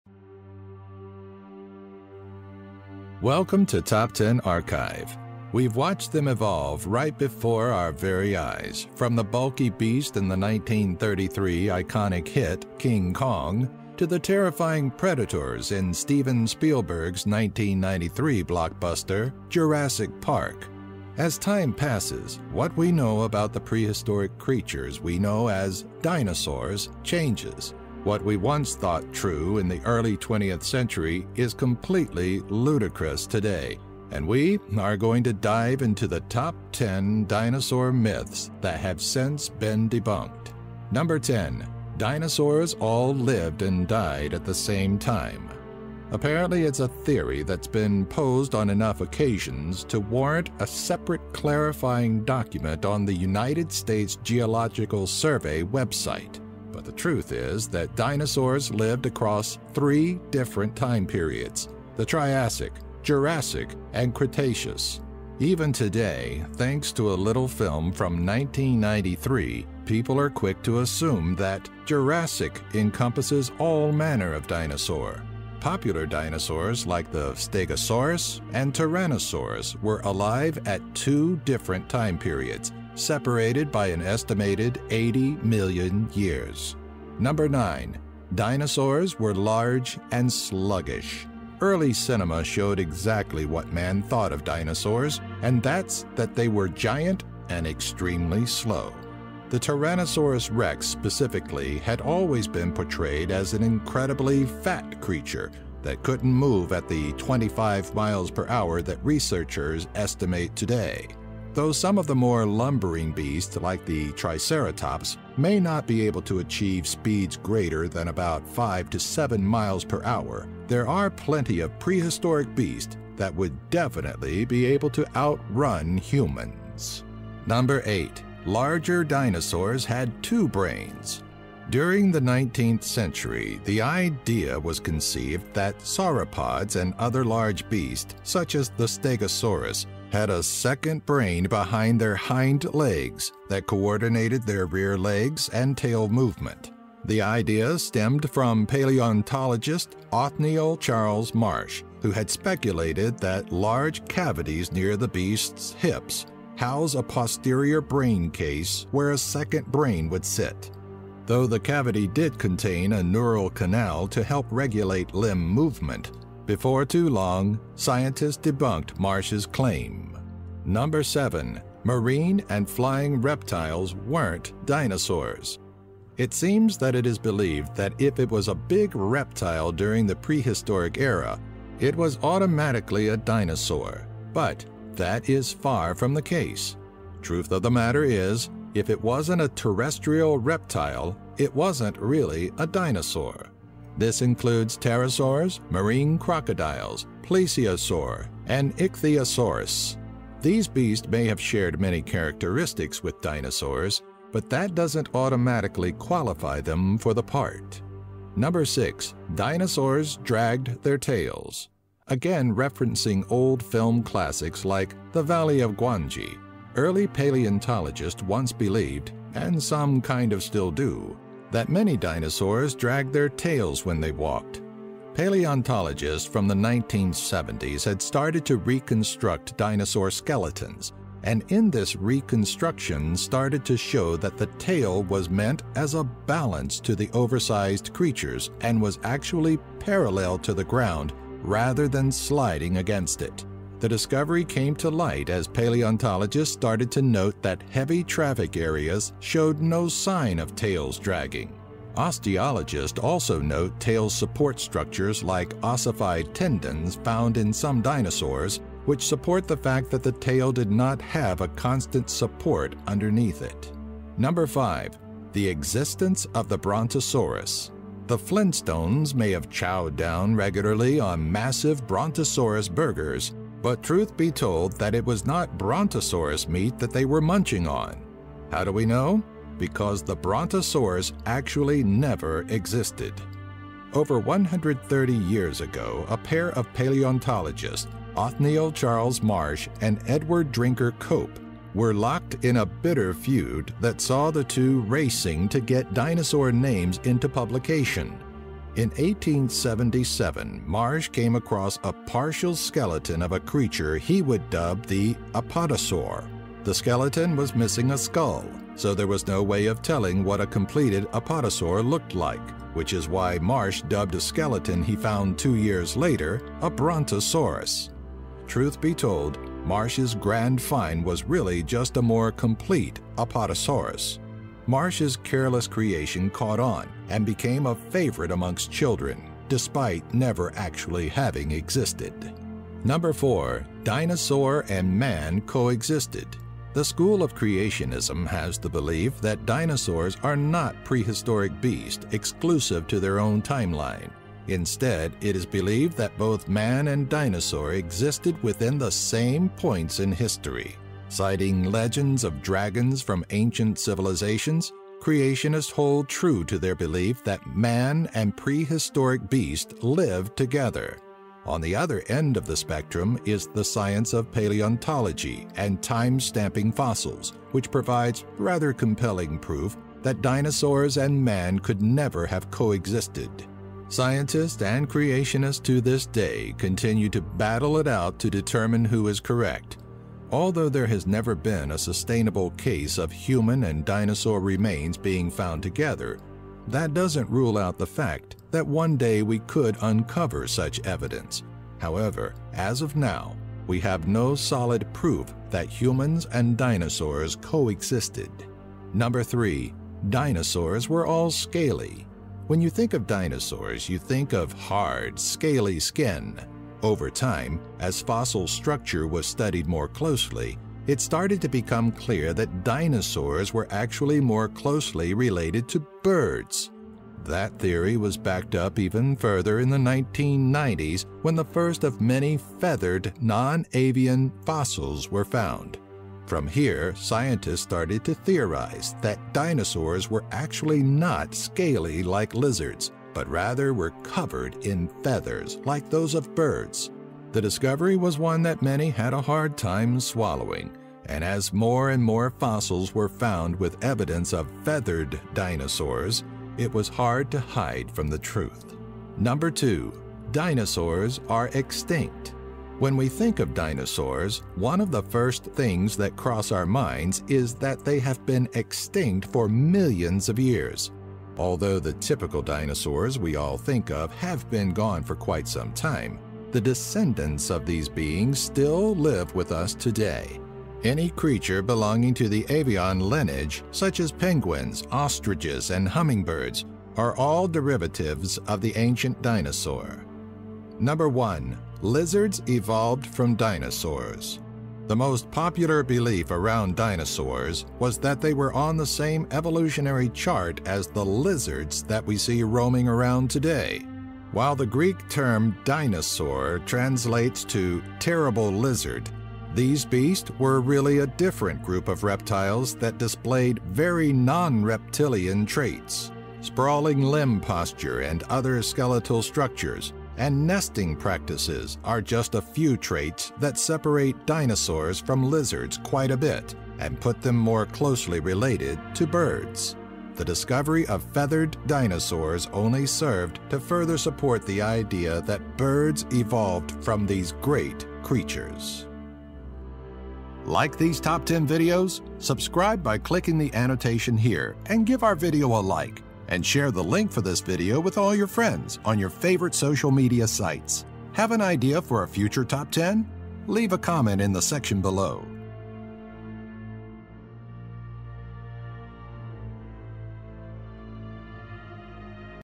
Top 10 Dinosaur Myths You Still Believe
Welcome to Top10Archive! We’ve watched them evolve right before our very eyes from the bulky beasts in the 1933 iconic hit King Kong to the terrifying predators in Stephen Spielberg’s 1993 blockbuster, Jurassic Park. As time passes, what we know about the prehistoric creatures we know as dinosaurs, changes. What we once thought true in the early 20th century is completely ludicrous today - and we are going to dive into the top 10 dinosaur myths that have since been debunked.

10. Dinosaurs All Lived and Died at the Same Time
9. Dinosaurs Were Large and Sluggish
8. Larger Dinosaurs Had Two Brains
7. Marine and Flying Reptiles Weren’t Dinosaurs
6. Dinosaurs Dragged Their Tails
5. The Existence of the Brontosaurus
4. Dinosaur and Man Coexisted
3. Dinosaurs Were All Scaly
2. Dinosaurs Are Extinct
1. Lizards Evolved from Dinosaurs